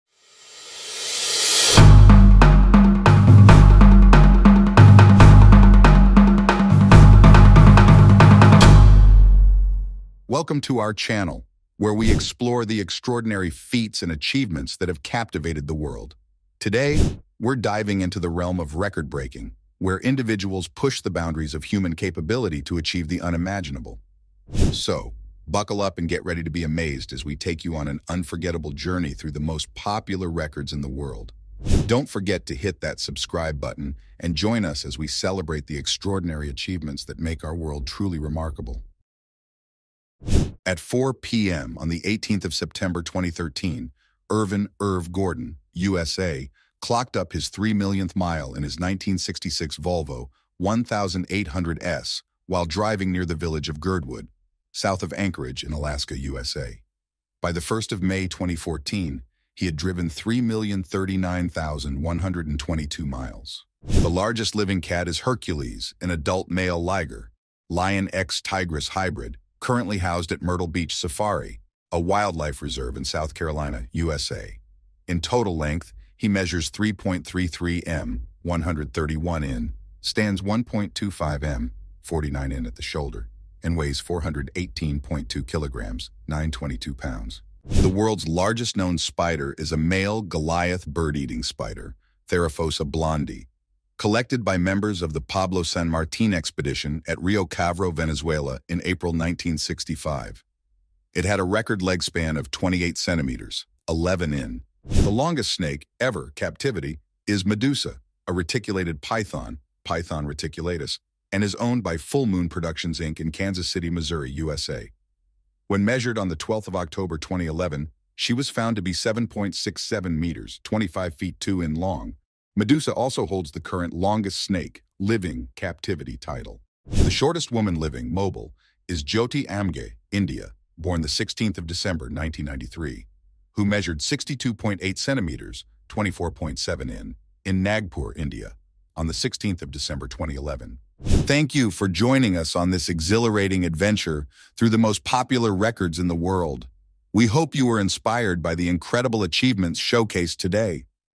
The Most Popular Records In The Word
Title: Exploring the Most Popular Records in the World | Guinness World Records

Description:
Get ready to embark on an exhilarating journey as we delve into the most popular records ever achieved in the world. From jaw-dropping feats of athleticism to mind-boggling displays of creativity and engineering marvels, this video will leave you in awe.

Join us as we showcase extraordinary achievements that have captured the hearts and minds of people across the globe. From the tallest skyscrapers to the fastest machines, we'll explore the astounding records that push the boundaries of human capability.

Discover the longest distances traveled, the largest structures ever built, and the most incredible talents on display. From the iconic Guinness World Records to lesser-known but equally impressive achievements, this video celebrates the remarkable accomplishments that make our world truly unique.

Whether you're a fan of extreme sports, engineering marvels, or simply enjoy witnessing the extraordinary, there's something for everyone in this compilation of the most popular records in the world.

Join us as we celebrate the spirit of human achievement and inspire others to dream big and reach for the stars.